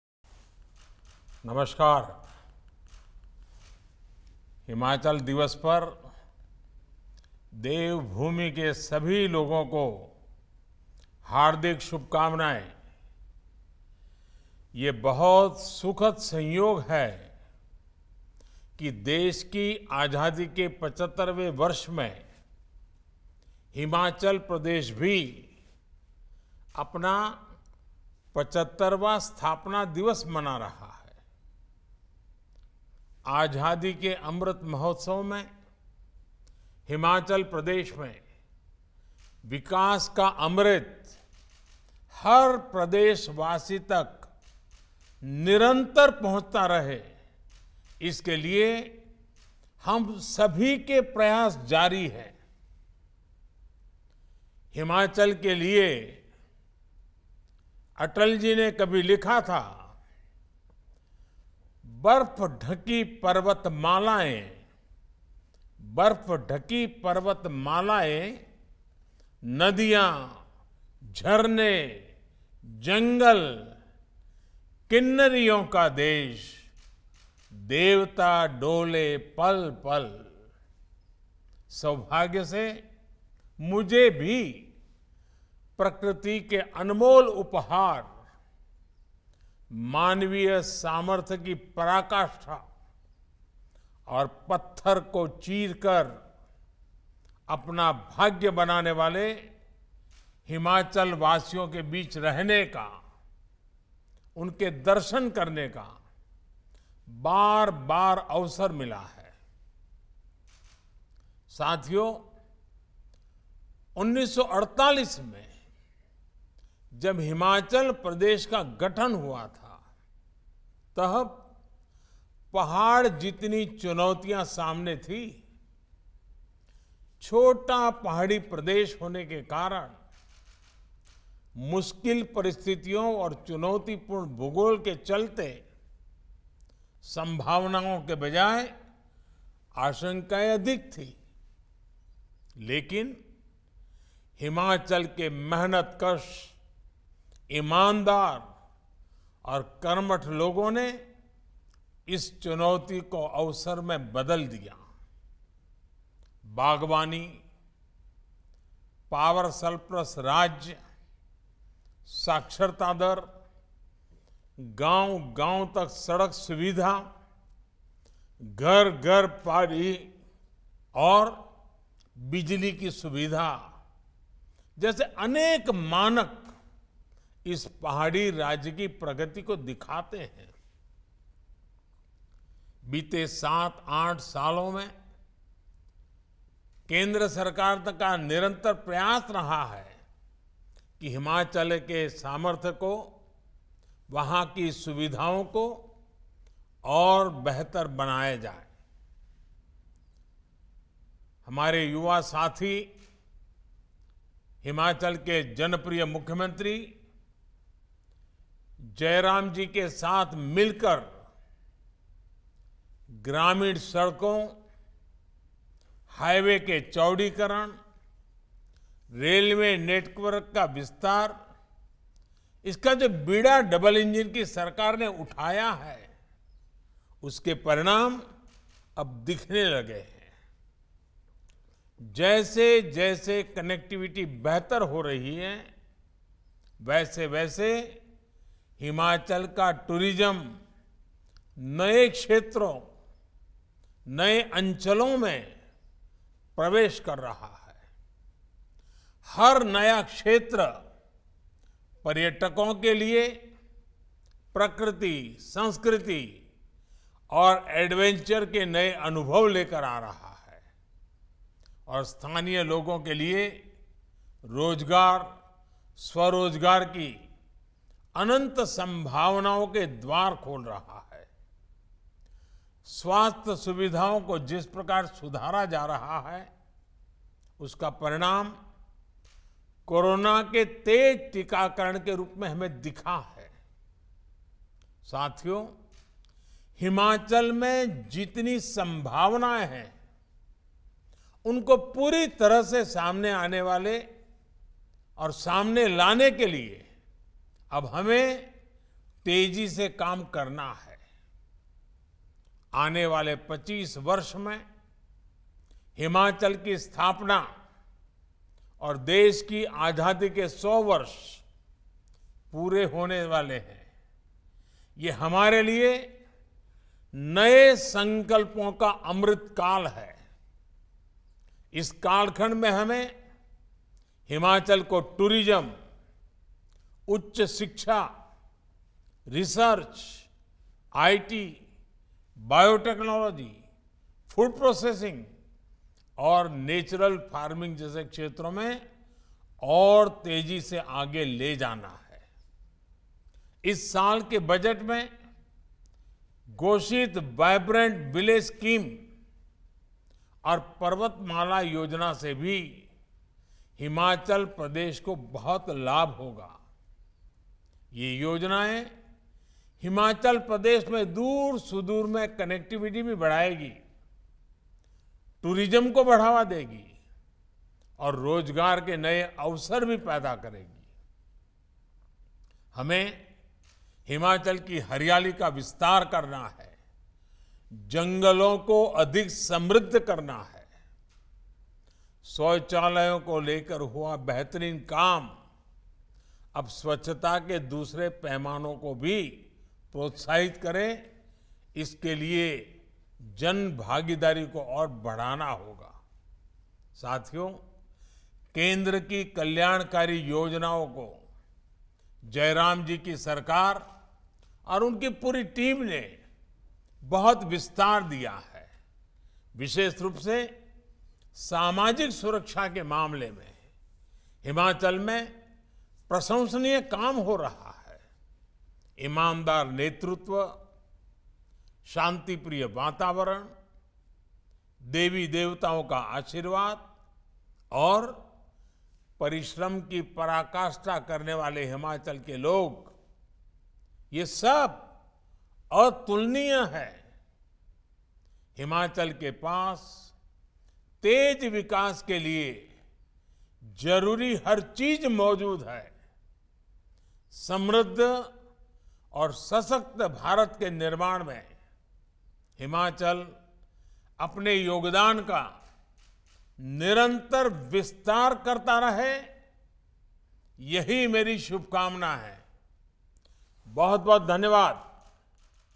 PM Modi's message on Himachal Divas (With Subtitles)
The Prime Minister, Shri Narendra Modi in his message, has greeted the people of Himachal Pradesh on the 75th establishment day and noted the happy coincidence that the 75th establishment day comes in the 75th Year of the Independence. He reiterated the commitment to take the nectar of development to every inhabitant of the state during the Azadi ka Amrit Mahotsav.